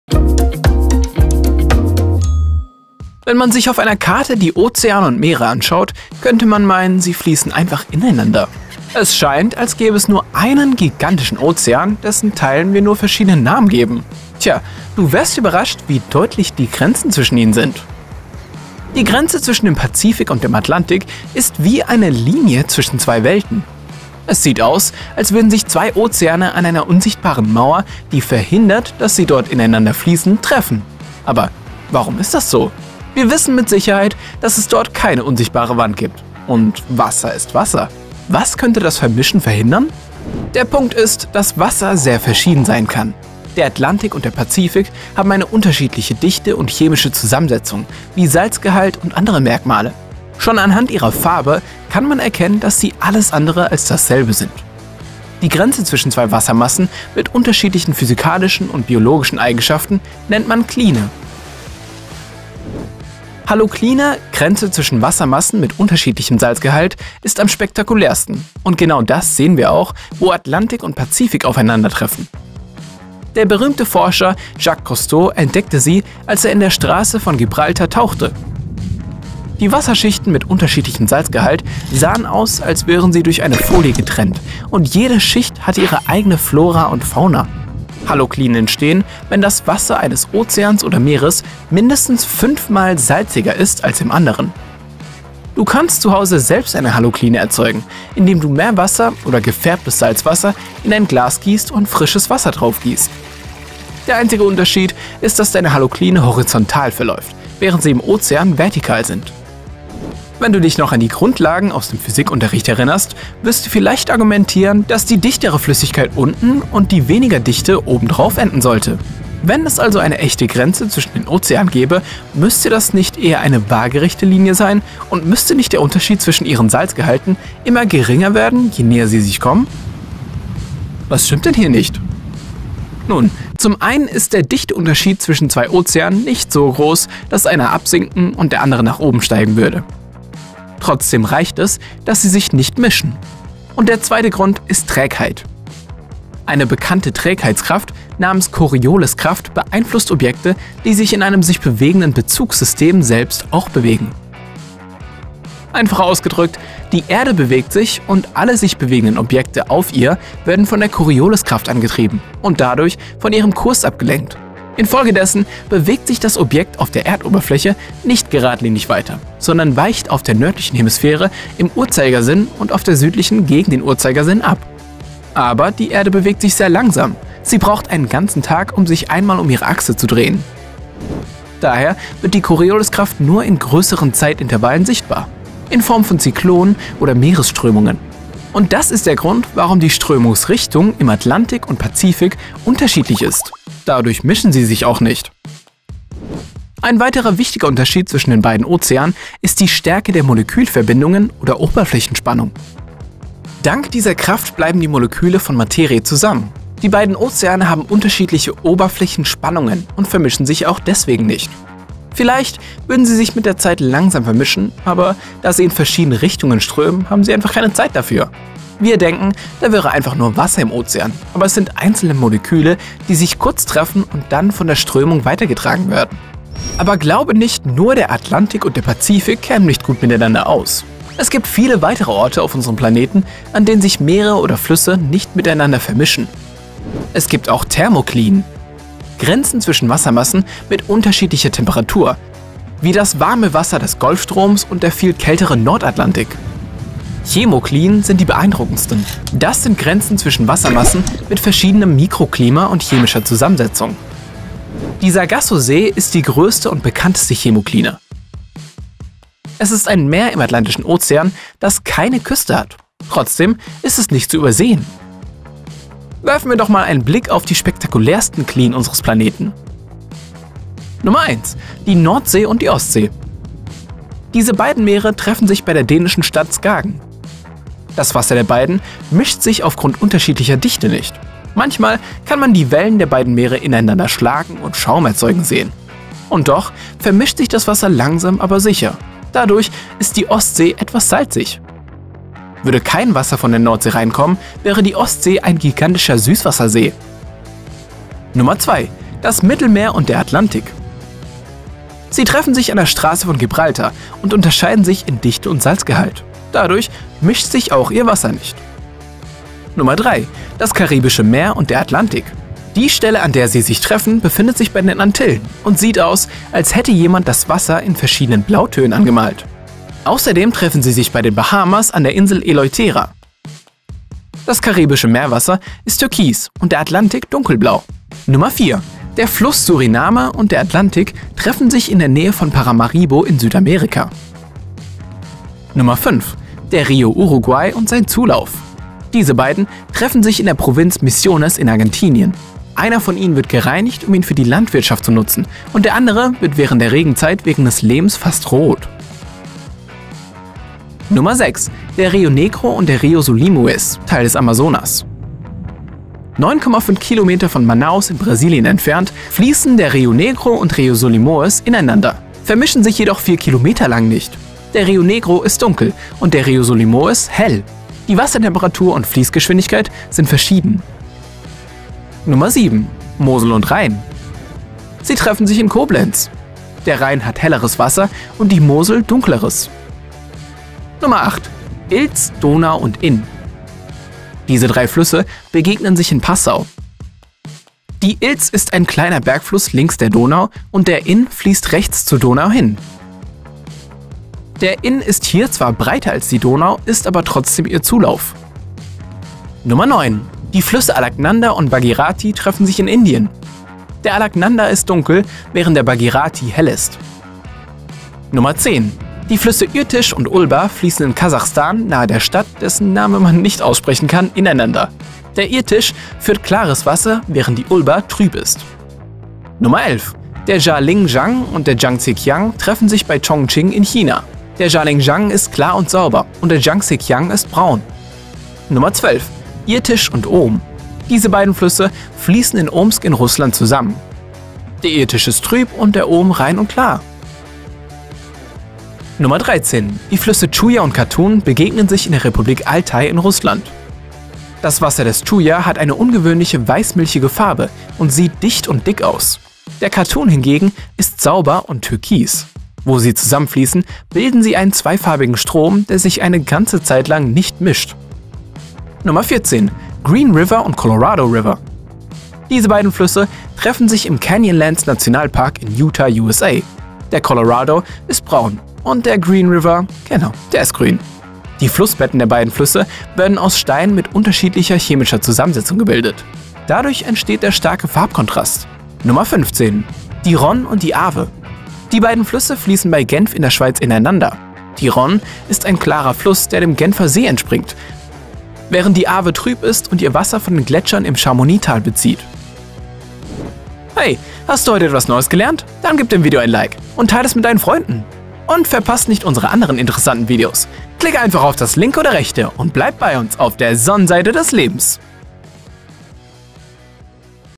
Warum sich zwei Ozeane nicht vermischen
Wenn man sich auf einer Karte die Ozeane und Meere anschaut, könnte man meinen, sie fließen einfach ineinander. Es scheint, als gäbe es nur einen gigantischen Ozean, dessen Teilen wir nur verschiedene Namen gegeben haben. Tja, du wärst überrascht, wie deutlich die Grenzen zwischen ihnen sind!
Die Grenze zwischen dem Pazifik und dem Atlantik ist wie eine Linie zwischen zwei Welten. Es sieht aus, als würden sich zwei Ozeane an einer unsichtbaren Mauer, die verhindert, dass sie ineinanderfließen, treffen. Aber warum ist das so? Wir wissen mit Sicherheit, dass es dort keine unsichtbare Wand gibt und Wasser ist Wasser. Was könnte das Vermischen verhindern?

ZEITMARKEN:
Wasser kann auch verschieden sein! 0:40
Was sind Halokline? 1:03
Die spektakulärsten Klinen unseres Planeten:
- Die Nordsee und die Ostsee 4:38
- Das Mittelmeer und der Atlantik 5:04
- Das karibische Meer und der Atlantik 5:16
- Der Fluss Suriname und der Atlantik 5:36
- Der Rio Uruguay und sein Zulauf 5:43
- Der Rio Negro und der Rio Solimões 5:58
- Mosel und Rhein 6:19
- Ilz, Donau und Inn 6:30
- Die Flüsse Alaknanda und Bhagirathi 6:48
- Irtysh und Ulba 6:56
- Der Jialing Jiang und der Jangtsekiang 7:11
- Irtysh und Om 7:20
- Die Flüsse Tschuja und Katun 7:20
- Green River und Colorado River 7:50
- Die Rhône und die Arve 8:10

ZUSAMMENFASSUNG:
- Der Atlantik und der Pazifik haben eine unterschiedliche Dichte und chemische Zusammensetzung, wie Salzgehalt und andere Merkmale. Schon anhand ihrer Farbe kann man erkennen, dass sie alles andere als dasselbe sind.
- Halokline – Grenze zwischen Wassermassen mit unterschiedlichem Salzgehalt – ist am spektakulärsten, und genau das sehen wir auch, wo Atlantik und Pazifik aufeinandertreffen.
- Haloklinen entstehen, wenn das Wasser eines Ozeans oder Meeres mindestens 5 Mal salziger ist, als im anderen.
- Ein weiterer wichtiger Unterschied zwischen den beiden Ozeanen ist die Stärke der Molekülverbindungen oder Oberflächenspannung.
- Vielleicht würden sie sich mit der Zeit langsam vermischen, aber da sie in verschiedene Richtungen strömen, haben sie einfach keine Zeit dafür.
- Es gibt auch Thermoklinen – Grenzen zwischen Wassermassen mit unterschiedlicher Temperatur, wie das warme Wasser des Golfstroms und der viel kältere Nordatlantik.
- Die Nordsee und die Ostsee treffen sich bei der dänischen Stadt Skagen. Das Wasser der beiden mischt sich aufgrund unterschiedlicher Dichte nicht.
- Das Mittelmeer und der Atlantik treffen sich an der Straße von Gibraltar und unterscheiden sich in Dichte und Salzgehalt. Dadurch mischt sich auch ihr Wasser nicht.
- Der Rio Uruguay und sein Zulauf treffen sich in der Provinz Misiones in Argentinien. Einer von ihnen wird gereinigt, um ihn für die Landwirtschaft zu nutzen und der andere wird während der Regenzeit wegen des Lehms fast rot.
- Mosel und Rhein treffen sich in Koblenz. Der Rhein hat helleres Wasser und die Mosel dunkleres.
- Alaknanda und Bhagirathi treffen sich in Indien. Der Alaknanda ist dunkel, während der Bhagirathi hell ist.
- Der Jialing Jiang und der Jangtsekiang treffen sich bei Chongqing in China. Der Jialing Jiang ist klar und sauber, und der Jangtsekiang ist braun.
- Das Wasser des Tschuja hat eine ungewöhnliche weiß-milchige Farbe und sieht dicht und dick aus. Der Katun hingegen ist sauber und türkis. Wo sie zusammenfließen, bilden sie einen zweifarbigen Strom, der sich eine ganze Zeit lang nicht mischt.
- Die Rhône und die Arve fließen bei Genf in der Schweiz ineinander. Die Rhône ist ein klarer Fluss, der dem Genfer See entspringt, während die Arve trüb ist und ihr Wasser von den Gletschern im Charmonixtal bezieht.